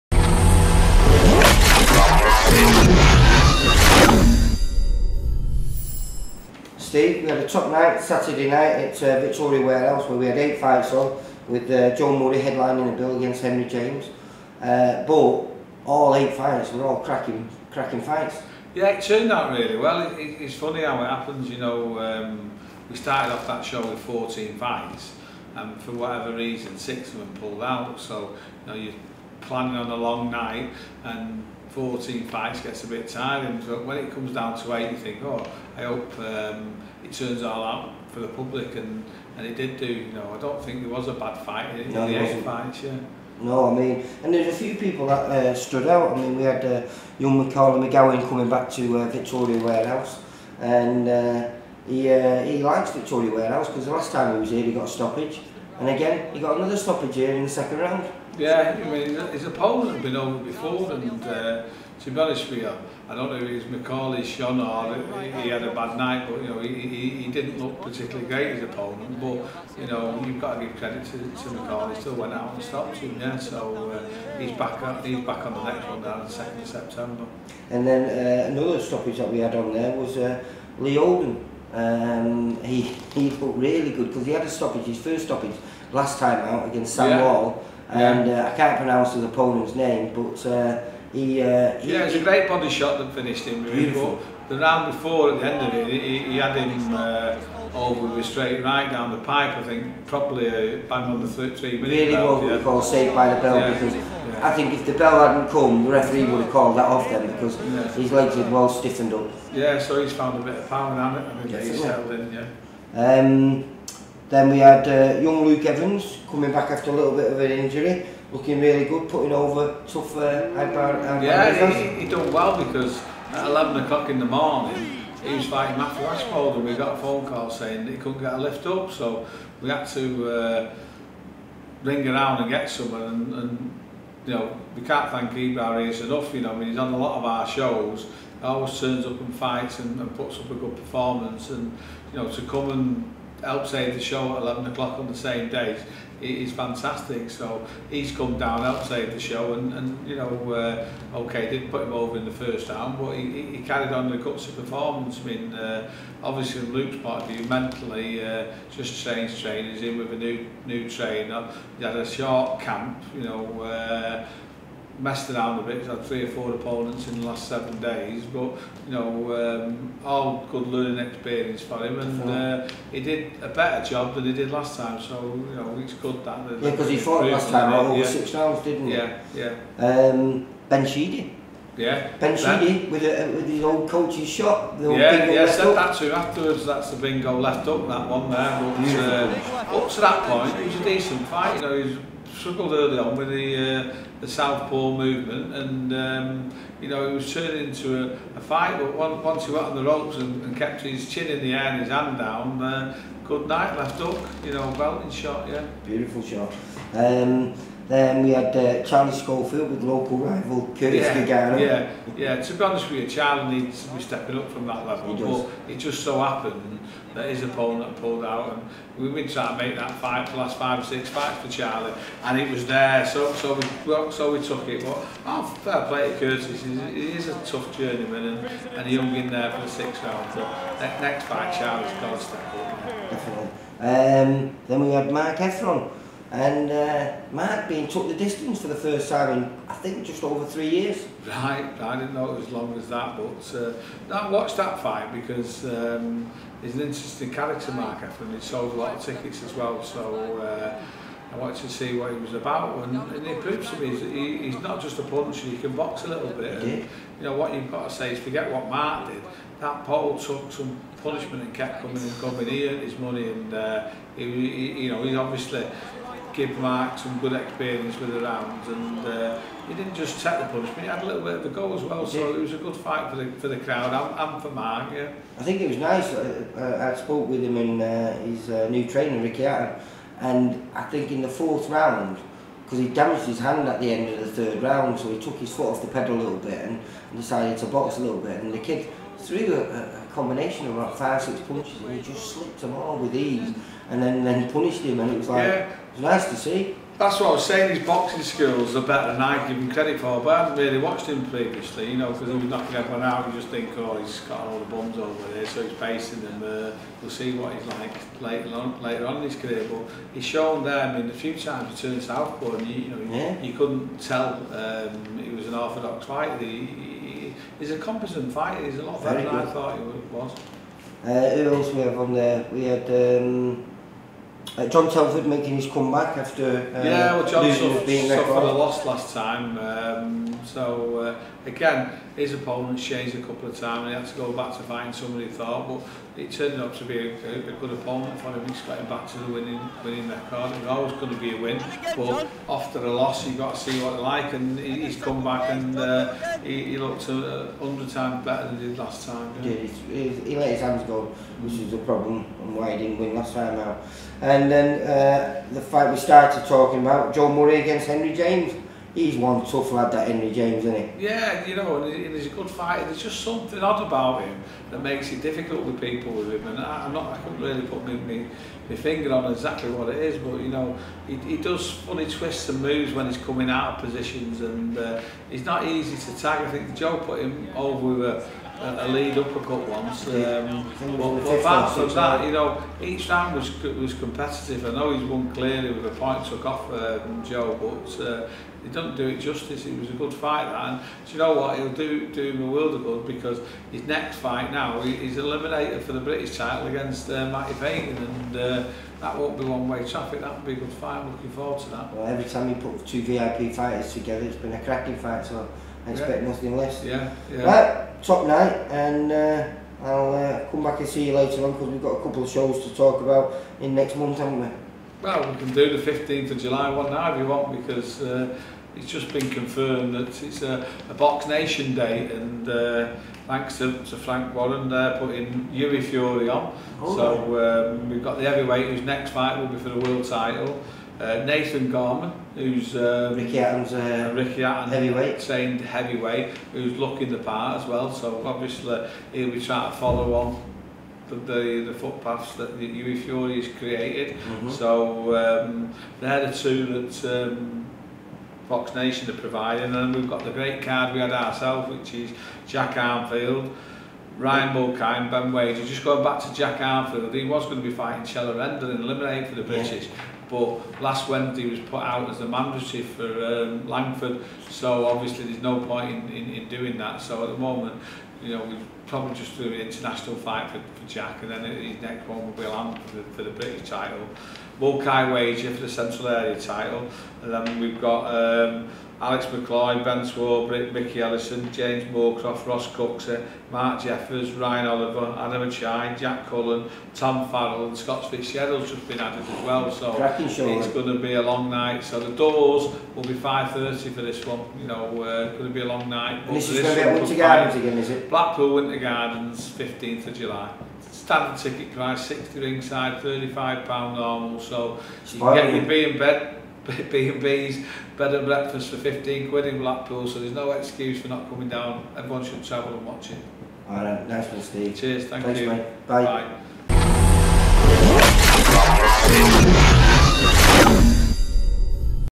Steve Wood talks about a great show at the Victoria Warehouse.
Steve Wood talks about a great show at the Victoria Warehouse last Saturday night, and about his next show in Blackpool on July 15th.